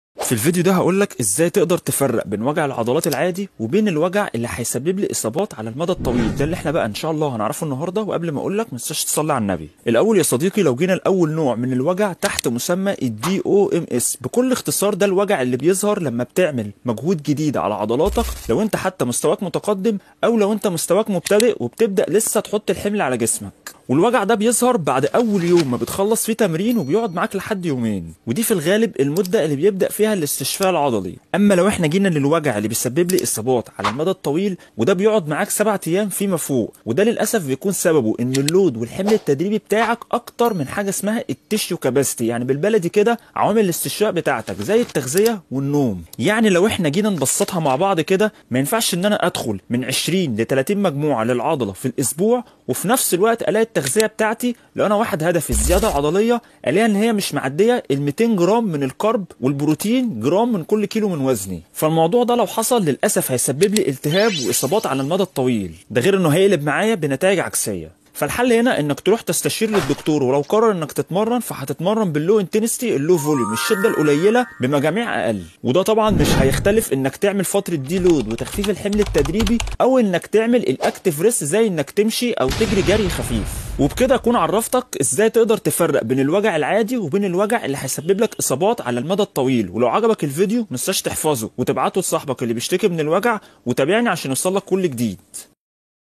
ازاي تفرق بين وجع العضلات و الآلم المسبب للإصابات
و لو عجبك الفيديو متنساش تعمل save+ follow و اكتبلي رأيك في التعليقات 💪🔥
و لو حابب اعملك نظام تدريبي و غذائي مخصصين ليك ابعتلي على الخاص بخصوص التدريب الاونلاين 📩